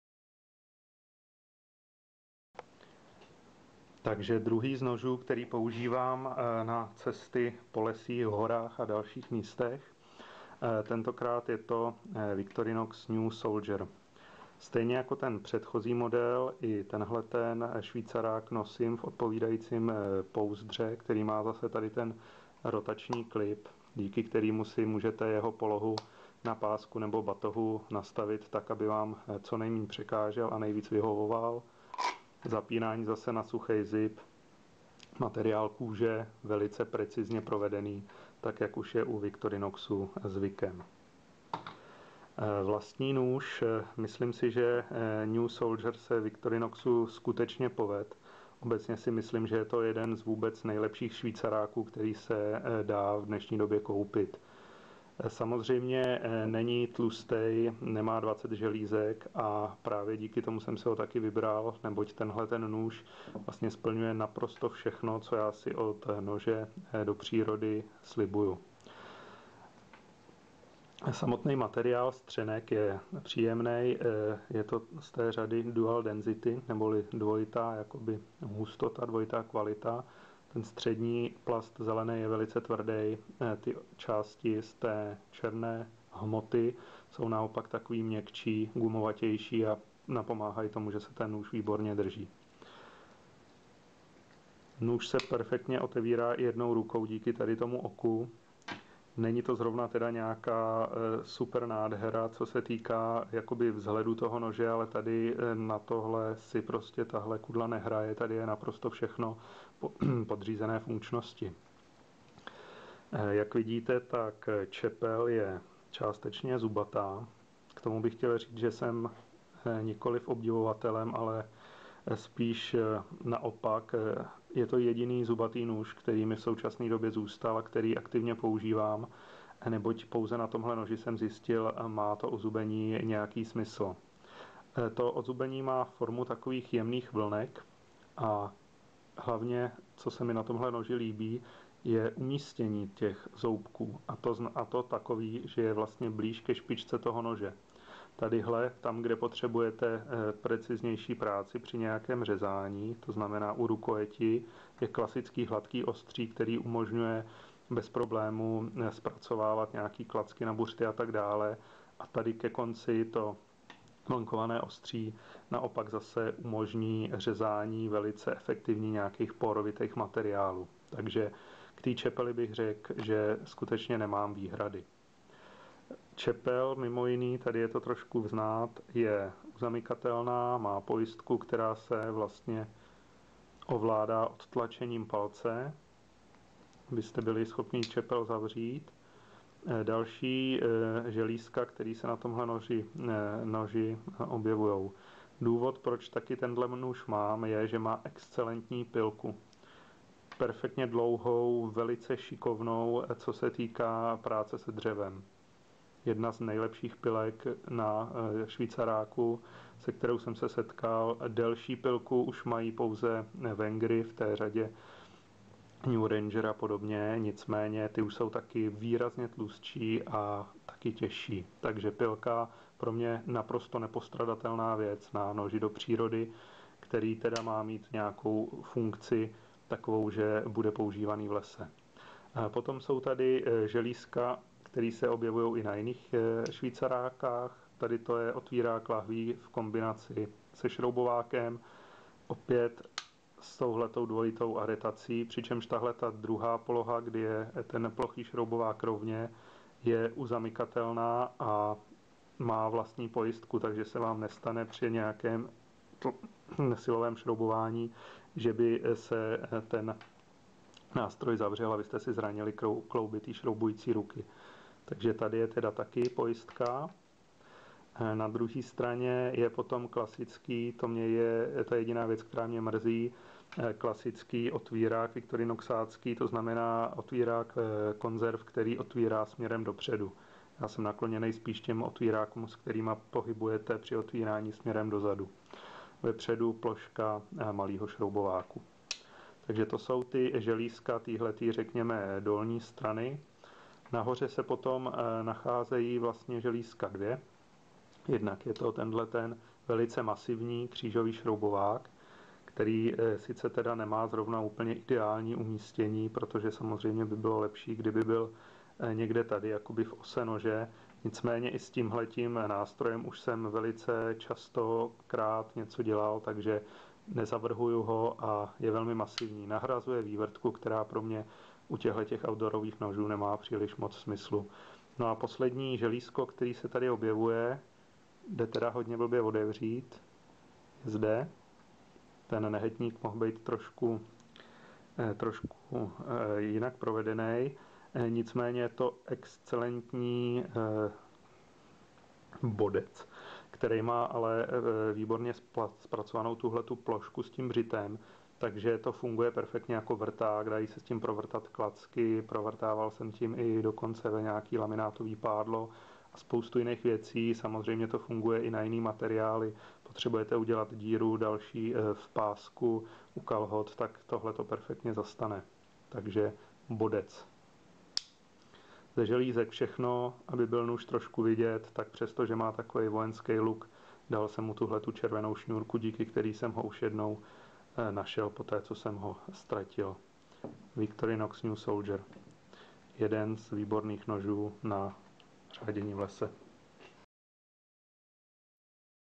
Vnox New Soldier
Větší "lesní" zavírák. Ideální vše v jednom na řádění v lese...
Bigger Forest Guy. All-in-one knife for outdoor and bushcraft or survival...